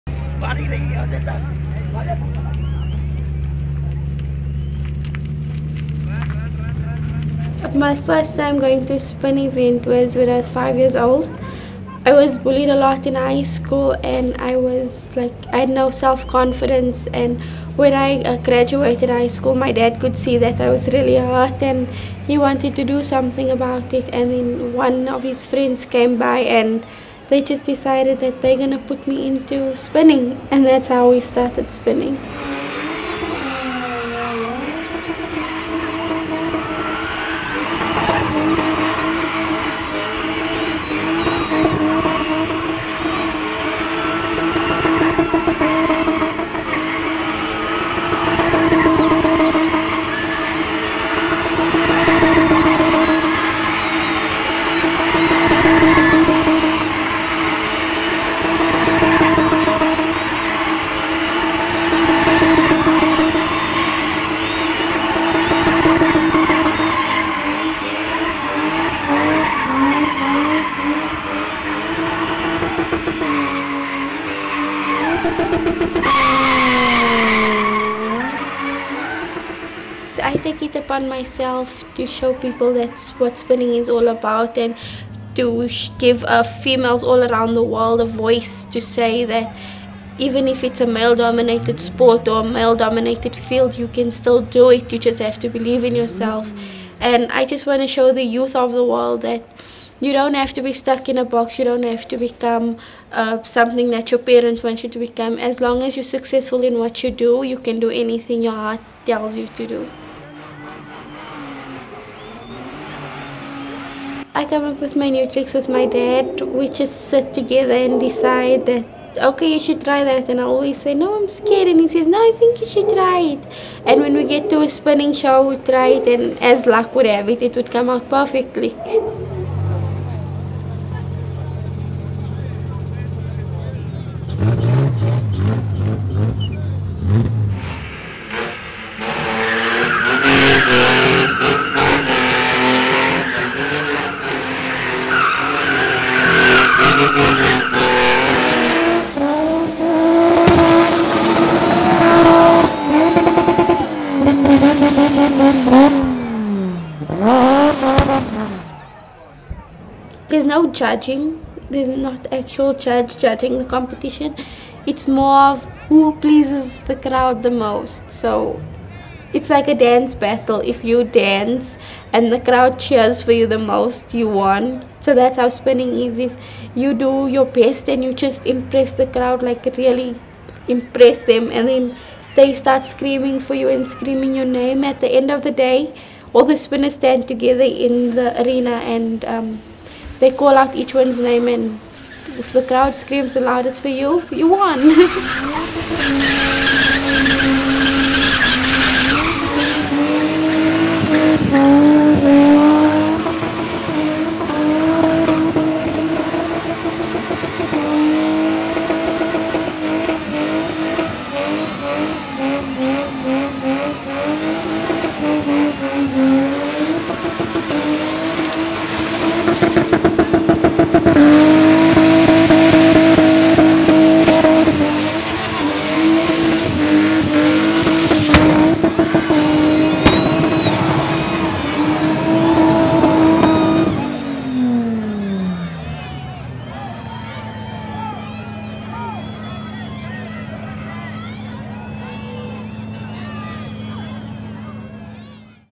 ‘Spinning’, Johannesburg, South Africa | Car subcultures
Around the world, communities have used cars in different and creative ways to express their own individual and group identities.

‘Spinning’ involves performing dangerous stunts, whilst a car – ideally a BMW 325i – is spun around in circles at high speed. The drivers lock the car into a spin and then, in clouds of smoke and squealing tyres, climbs out of the car or even hangs backwards out of the window with their head almost brushing the ground.

Spinning has its roots in the early 1980s in Soweto, Johannesburg, where gangsters and criminals would steal cars to spin at the funerals of dead friends. It is now the fastest growing motor sport in South Africa with a new generation pushing to make it mainstream.

Stacey-lee May, AKA ‘Queen of Smoke’, is a 23-year-old law student and one of South African’s top female motorsport sensations. Her introduction to spinning came from being bullied in High School, and at the age of 16, her father decided to teach her how to spin in order to boost her confidence.

“I just want to show the youth of the world that you don’t have to be stuck in a box…you can do anything you heart tells you to do”.

This film was produced as part of Cars: Accelerating the Modern World (23 November 2019 – 19 April 2020)